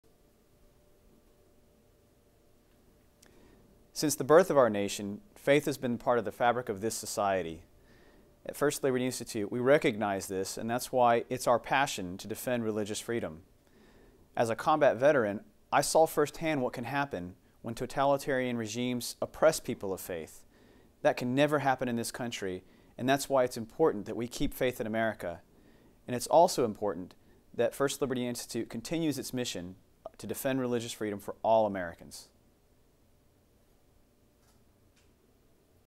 Mike Berry at First Liberty Institute Discusses Keeping Faith in America
Mike Berry, Deputy General Counsel and Director of Military Affairs with First Liberty​ Institute discusses the importance of Keeping Faith in America!

First Liberty Institute is the largest legal organization in the nation dedicated exclusively to protecting religious liberty for all Americans.